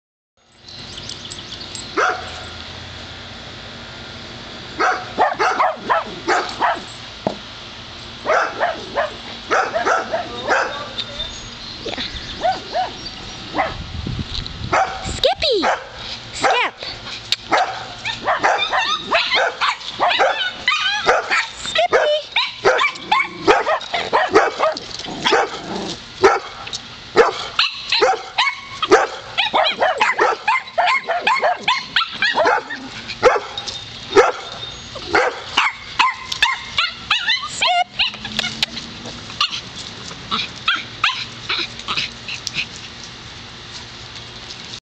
Skippy in backyard
This was the first time we took Skippy to run around my parents backyard and meet the other neighborhood dogs.